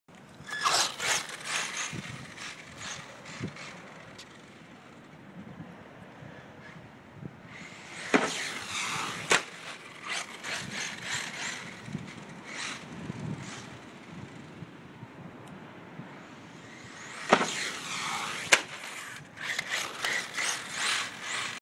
HobbyTown Sunday. Hpi Savage XS jumping
Messing around at HobbyTown Orland Park.